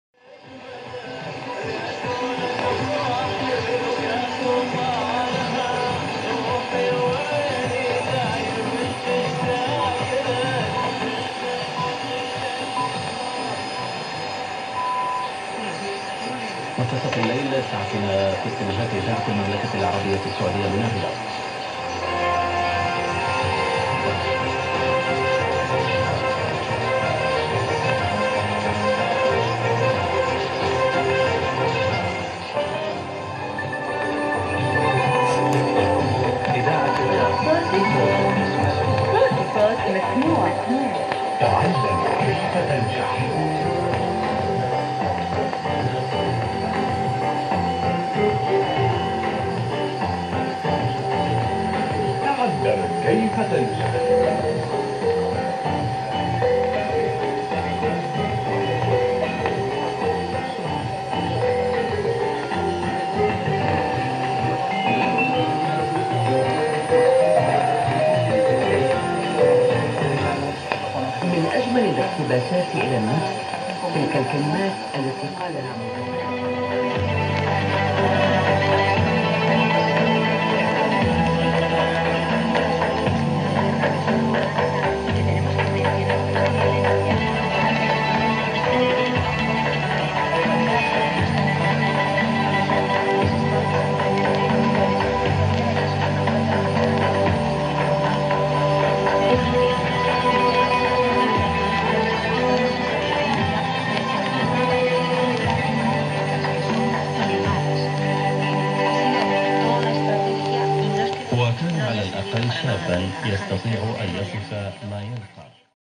Saba Lindau: 1521 Khz Radio Riyadh Saudi Arabia
06/02/2015  21:00 UTC
1521 Khz SBC Radio Riyadh received in Germany
Saba Lindau with build- in ferrite antenna